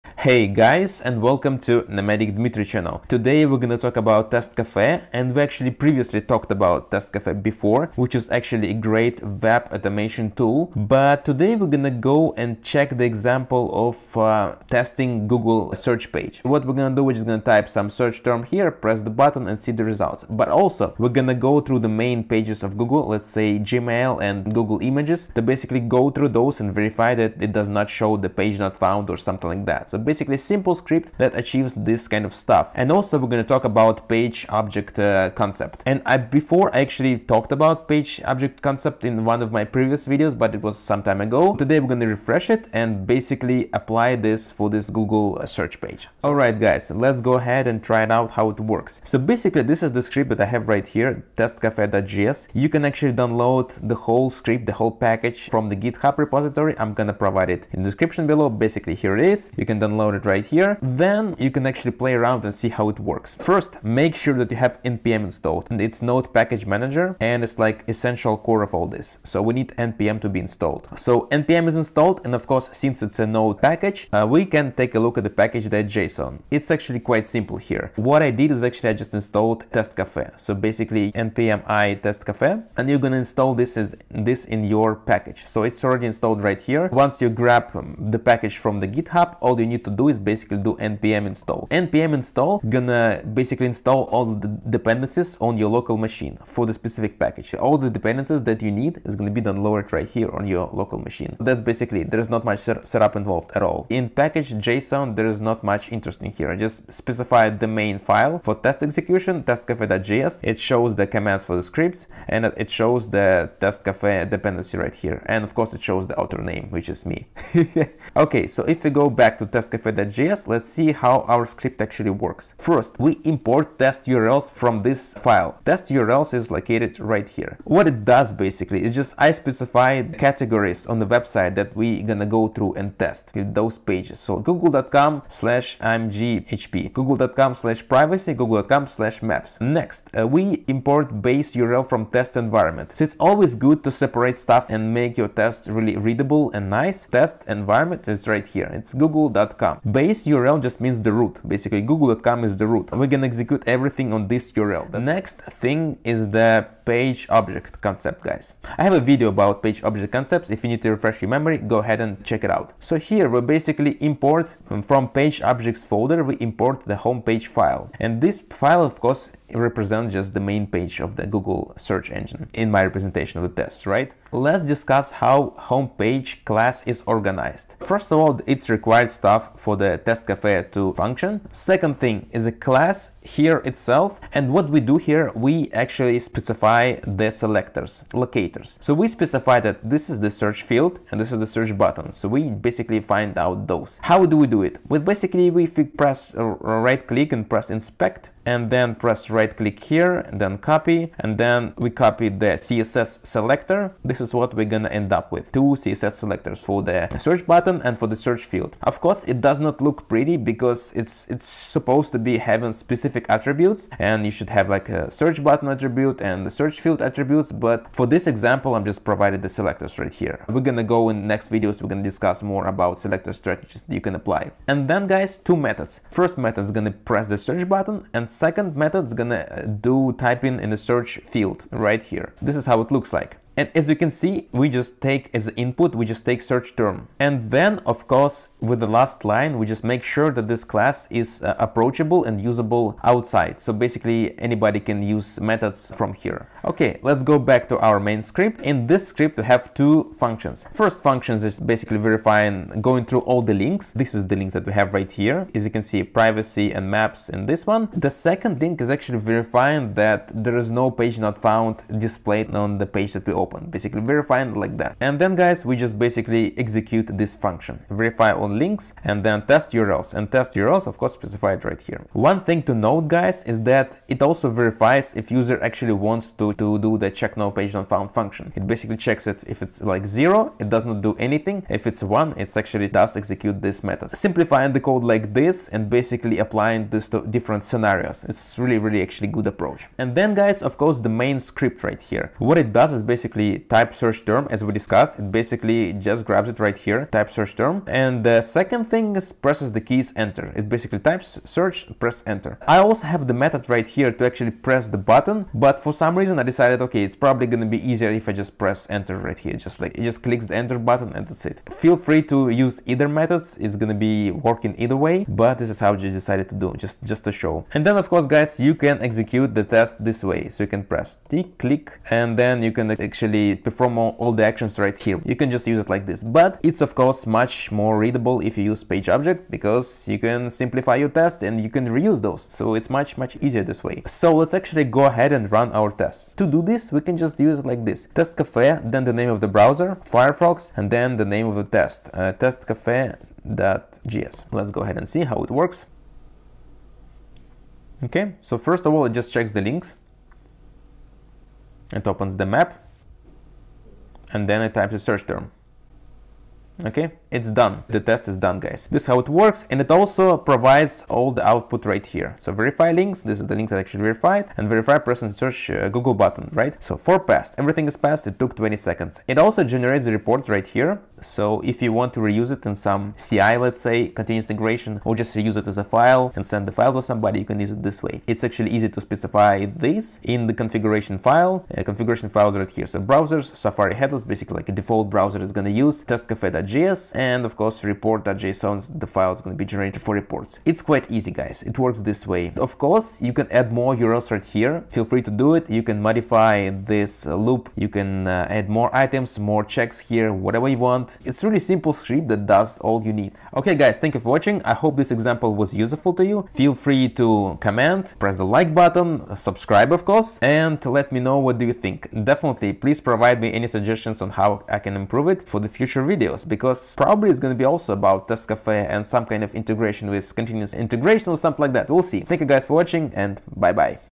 TestCafe. Automating Google and Checking Links. Page-Object Model
Today we are talking about TestCafe Automation tool and specific concepts:
- Page-Object Model
- Verifying links on the Website

📚My Medium articles:

🎴Relax. Travel doesn't solve any of your problems
💻Android Automation. Simple Espresso tutorial